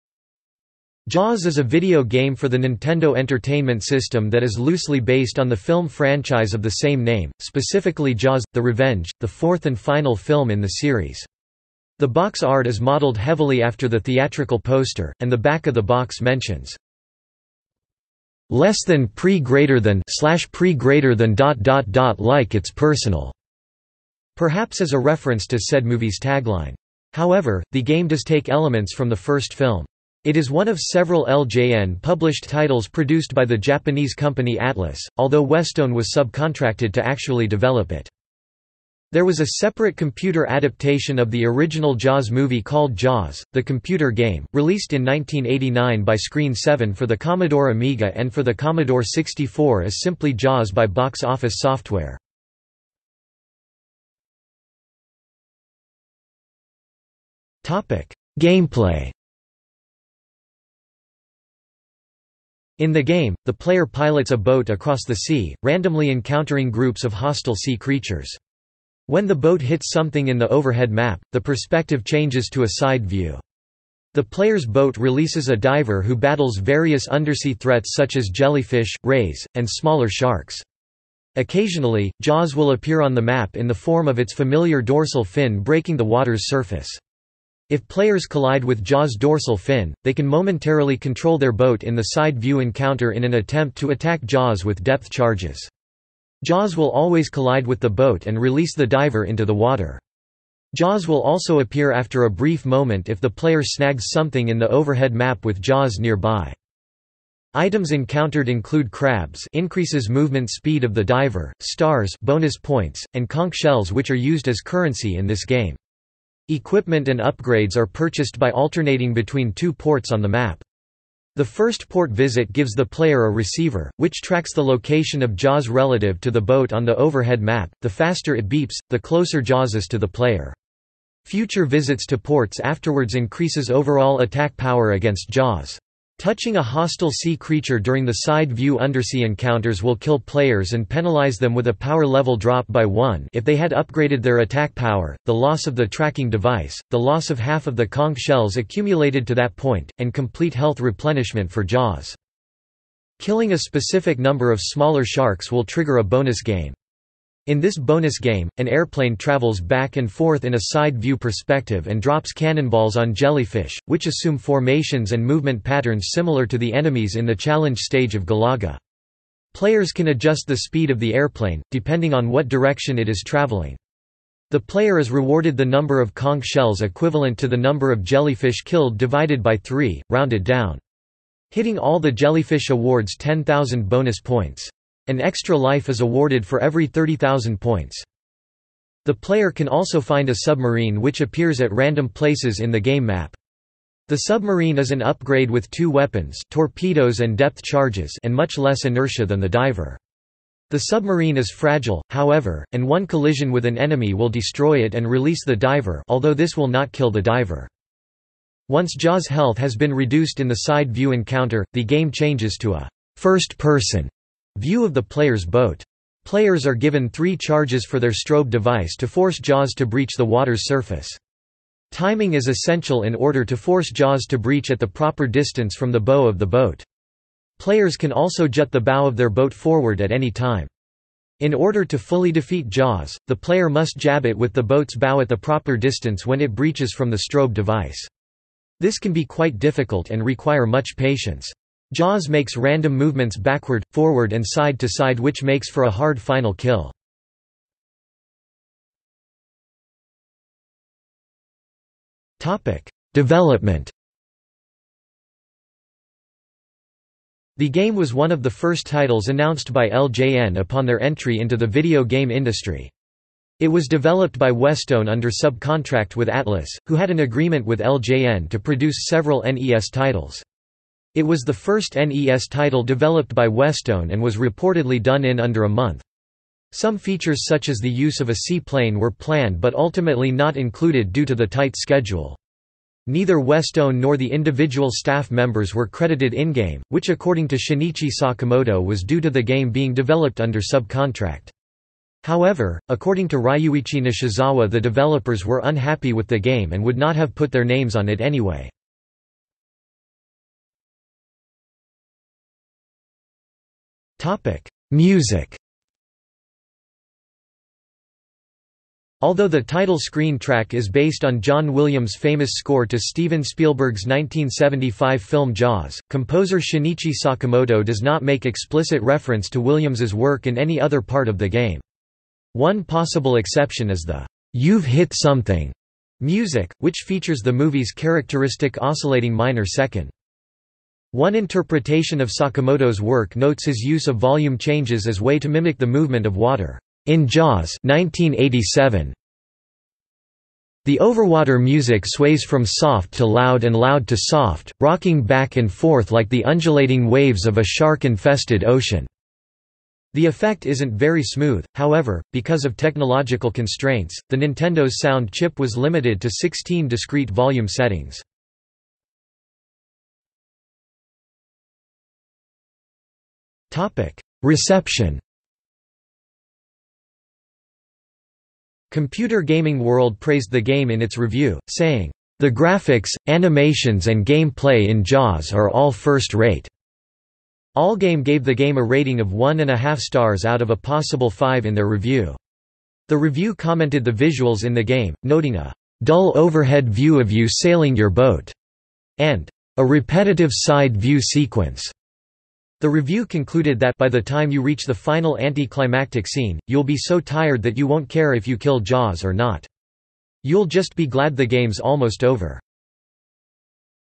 Jaws (video game) | Wikipedia audio article
00:00:54 1 Gameplay
00:04:08 2 Development
00:05:00 3 Music
00:06:00 4 Reception

- Socrates

SUMMARY
Jaws is a video game for the Nintendo Entertainment System that is loosely based on the film franchise of the same name, specifically Jaws: The Revenge, the fourth and final film in the series. The box art is modeled heavily after the theatrical poster, and the back of the box mentions "...like it's personal", perhaps as a reference to said movie's tagline. However, the game does take elements from the first film. It is one of several LJN-published titles produced by the Japanese company Atlus, although Westone was subcontracted to actually develop it.
There was a separate computer adaptation of the original Jaws movie called Jaws: The Computer Game, released in 1989 by Screen 7 for the Commodore Amiga and for the Commodore 64 as simply Jaws by Box Office Software.